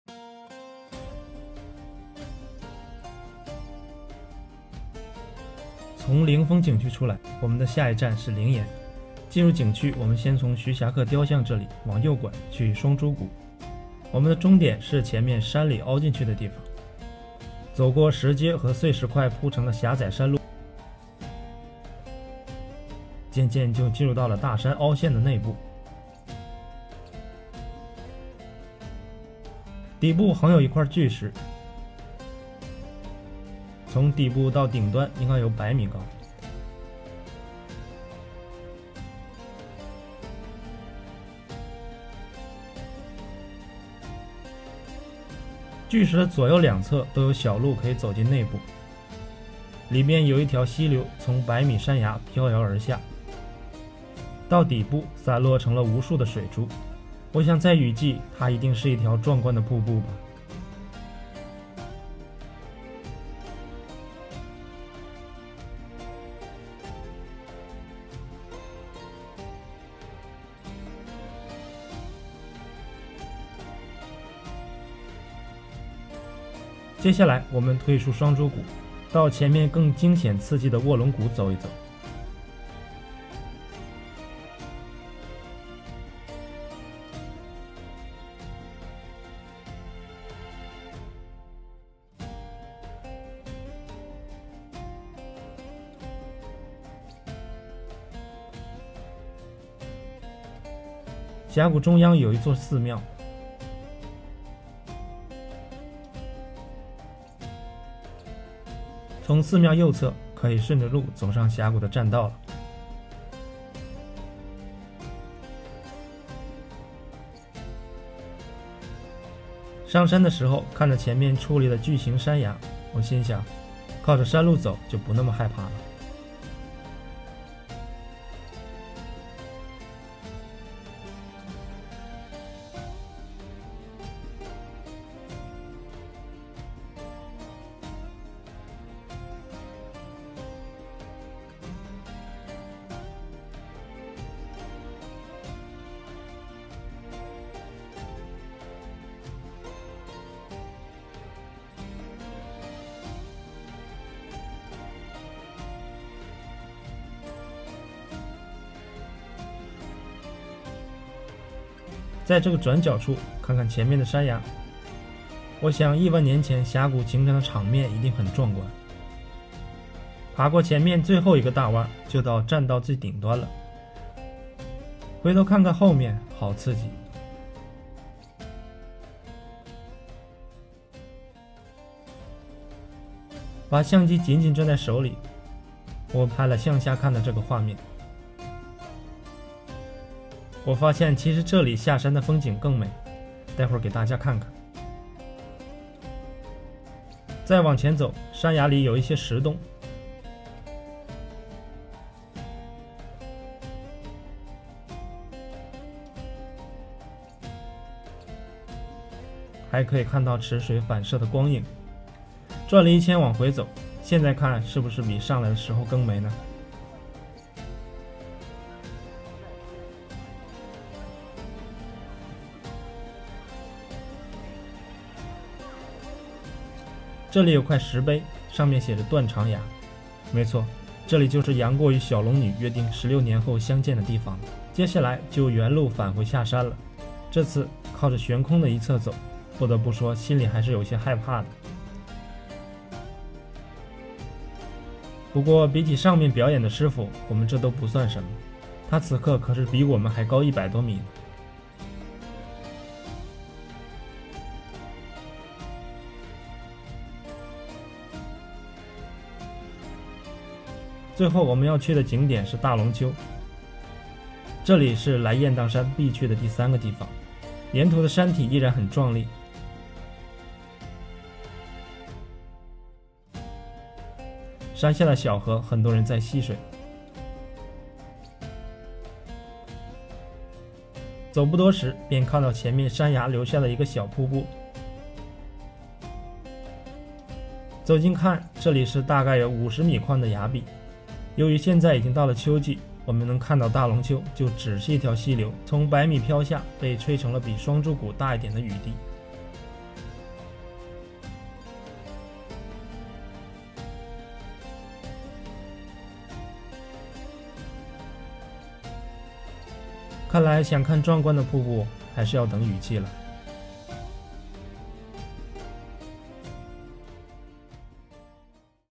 【Travel in China中国旅行纪实-浙江温州雁荡山】走进雁荡山百米卧龙谷，没想到意外发现此处就是杨过跳下去断肠崖
雁荡山，简称雁山、雁岩，素有“海上名山、寰中绝胜”“东南第一山”之誉，雁荡山形成于一亿二千万年以前。火山喷发造化了雁荡山雄奇壮丽的景观。